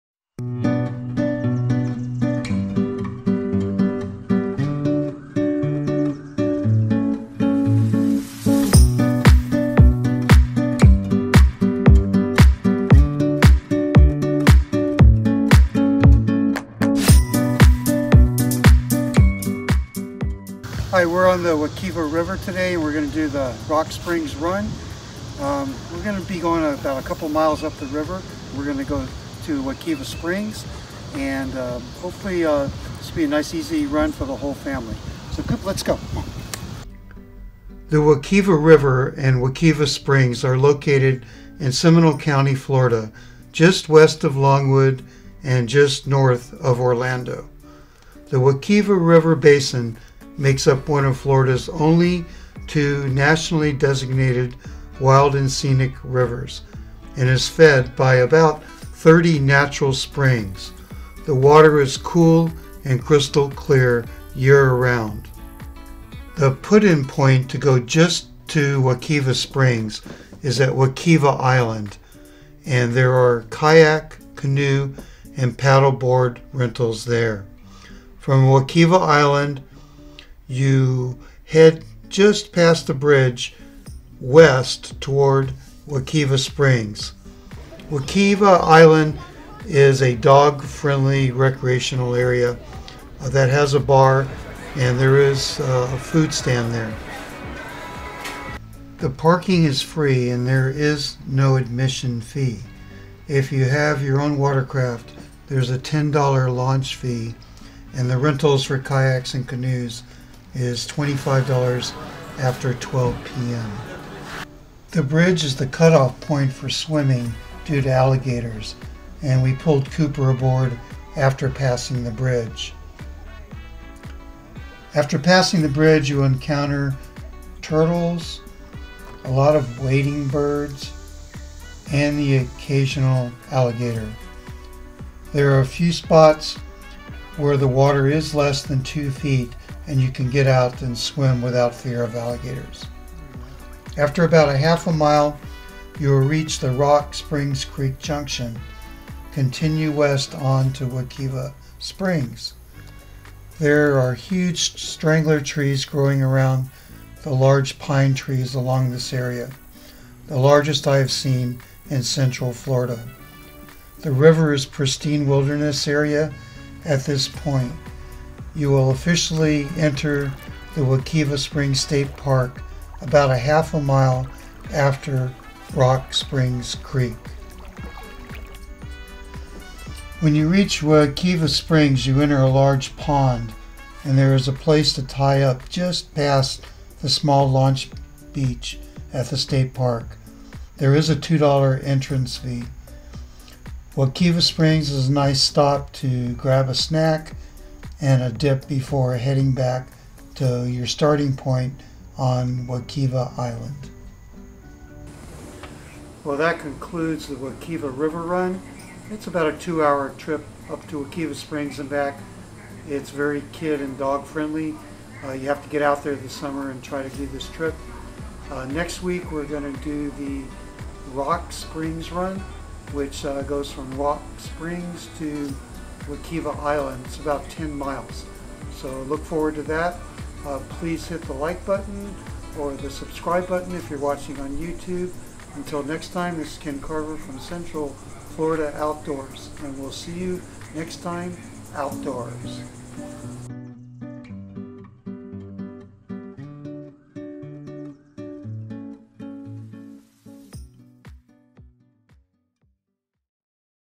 Wekiva River Run
An easy 2 hour trip up the National  Designated Wild and Scenic Wekiva River, just north of Orlando through pristine wilderness.